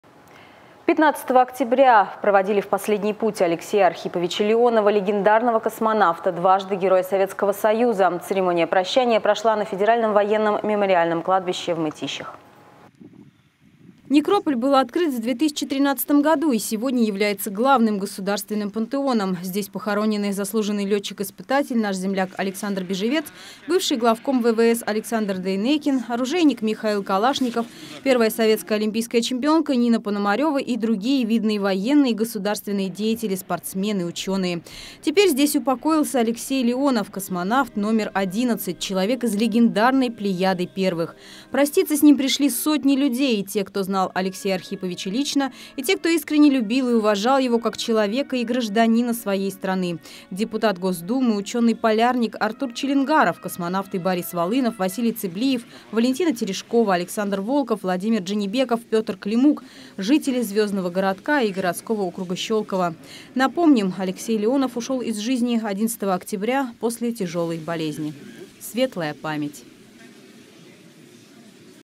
Прощание с Алексеем Леоновым
В последний путь проводили Алексея Архиповича Леонова, легендарного космонавта Первого отряда, Дважды Героя Советского Союза. Церемония прощания прошла на Федеральном военном мемориальном кладбище в Мытищах.